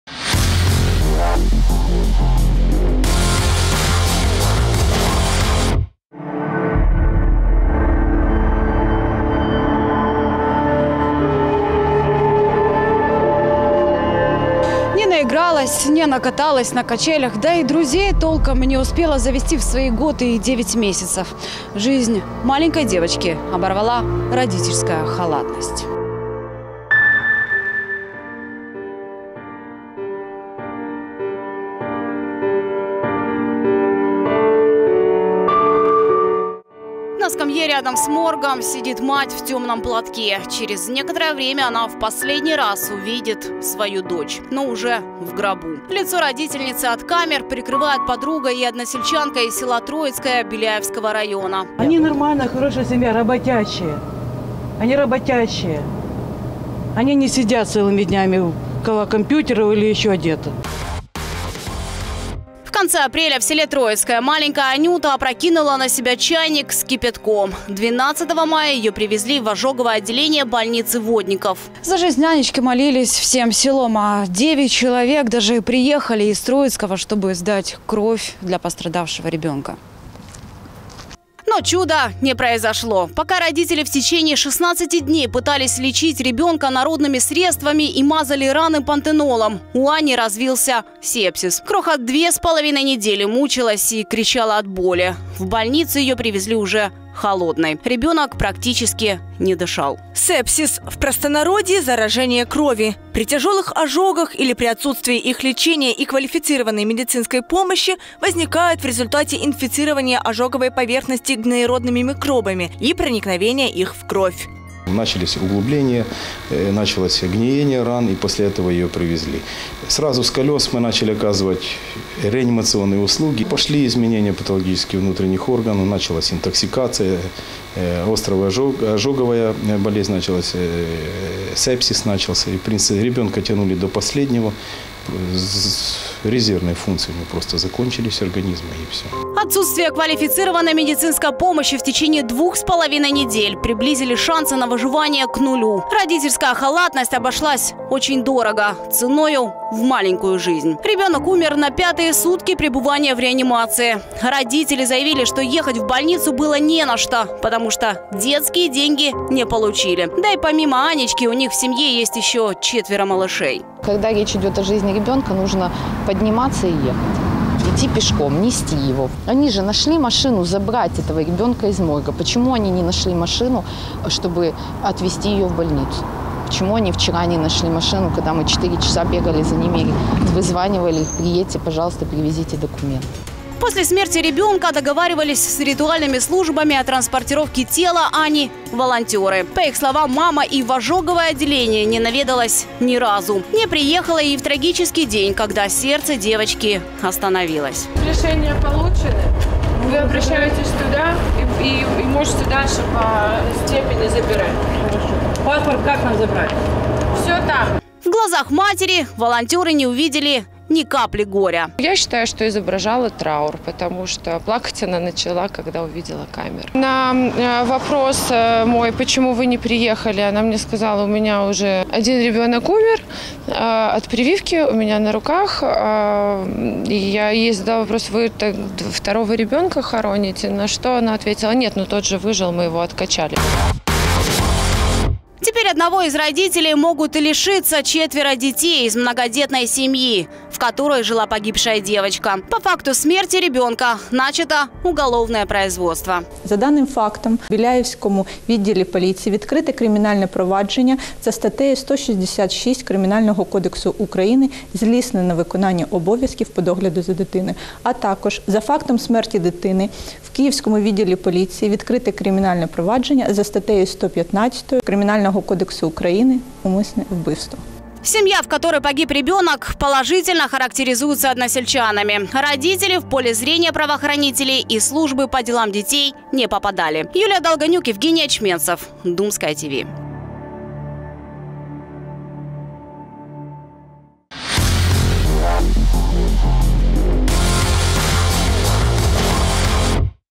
Авторский репортаж: родительская халатность ценою в детскую жизнь
На скамье рядом с моргом сидит мать в темном платке. Через некоторое время она в последний раз увидит свою дочь. Но уже в гробу.